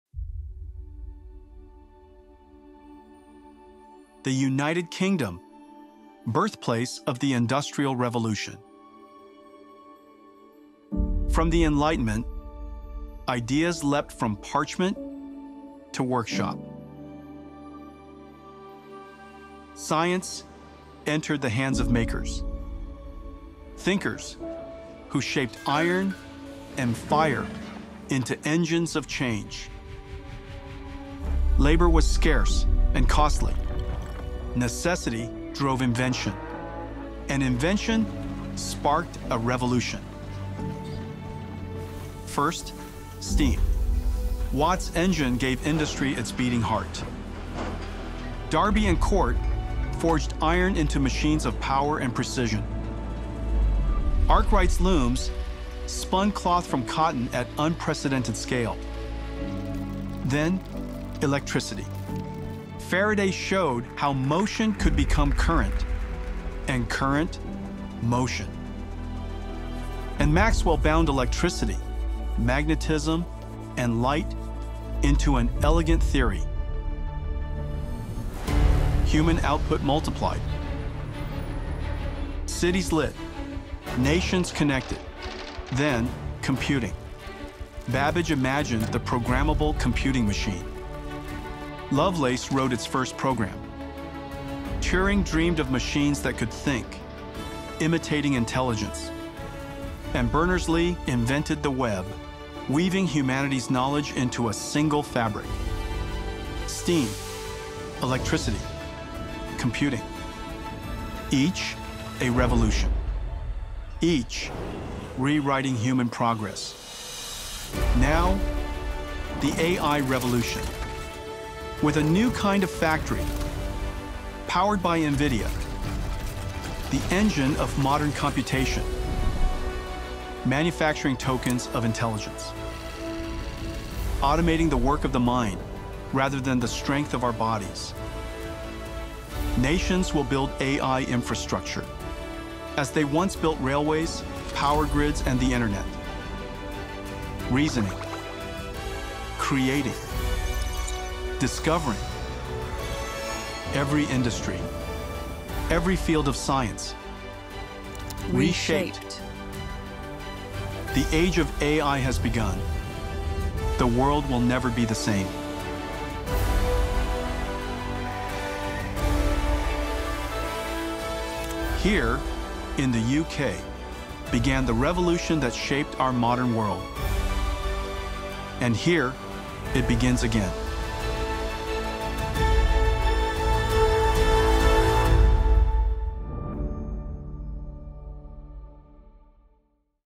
The U.K.'s Next Industrial Revolution
The UK sparked the first industrial revolution. Now it’s launching the next—powered by NVIDIA AI infrastructure.

Explore how AI is reshaping every industry—from services and life sciences, to quantum and robotics, manufacturing, and energy.